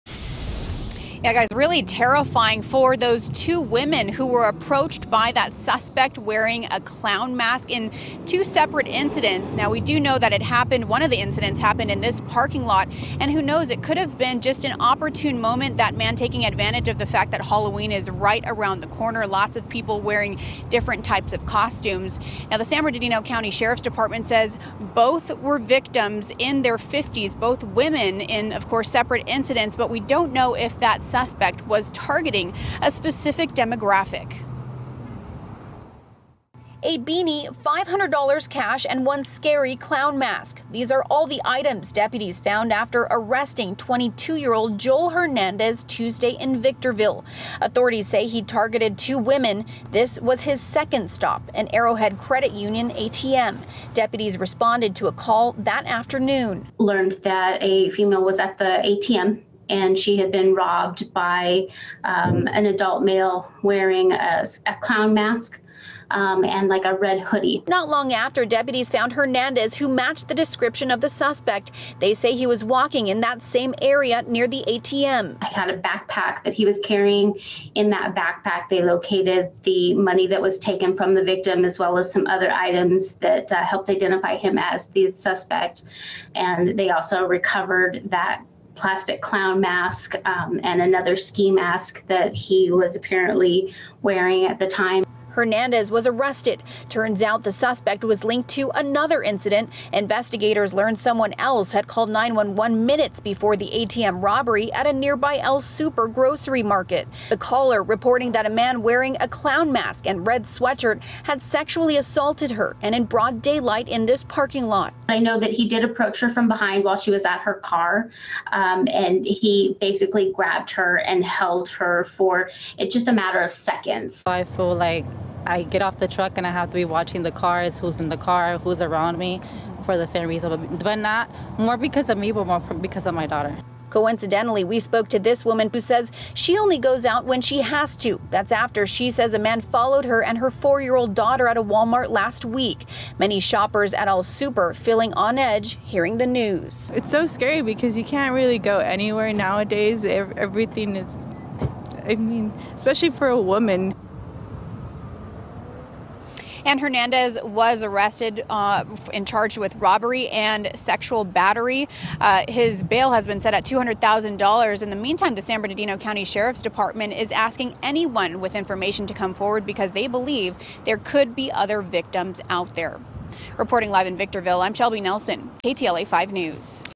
Man wearing clown mask attacks 2 women in Victorville: Police
A man was arrested after allegedly sexually assaulting one woman and robbing another in Victorville while wearing a clown mask, officials said Wednesday.

Shelby Nelson reports for the KTLA 5 News at 6 on Oct. 26, 2022.